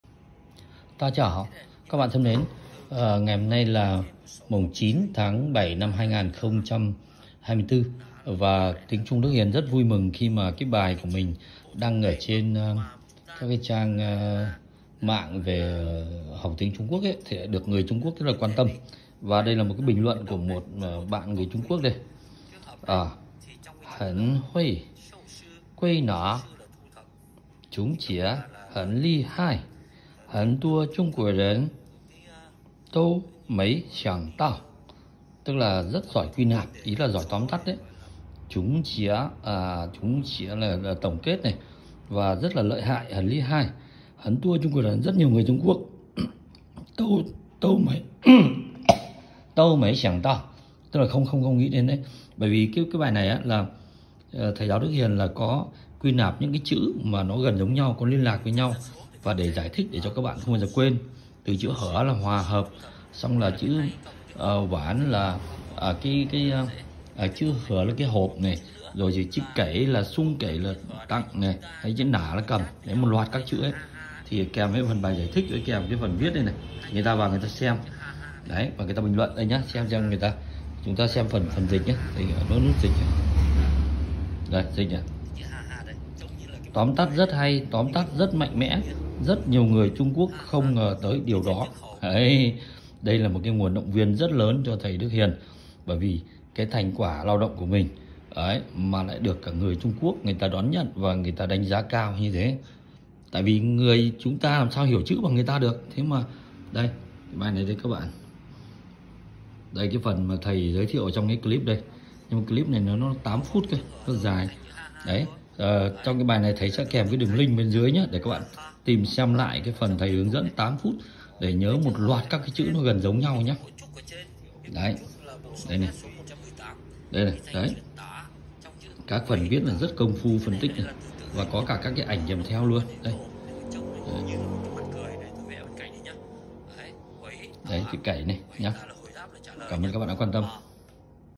Người Trung Quốc đánh giá các bài luận Hán văn của thầy giáo Đức Hiền như thế nào?
Người Trung Quốc đánh giá rất cao những bài giảng giải thích logic chữ Hán của thầy giáo Đức Hiền￼. Họ cmt:
- Rất giỏi quy nạp, tổng kết. Thật lợi hại, mạnh mẽ. Rất nhiều người Trung Quốc không ngờ tới điều đó !
很会归纳总结， 很厉害， 很多中国人都没想到
2024年7月9号
Bài gốc Luận Hán Văn có cmt của người Trung quốc đây ạ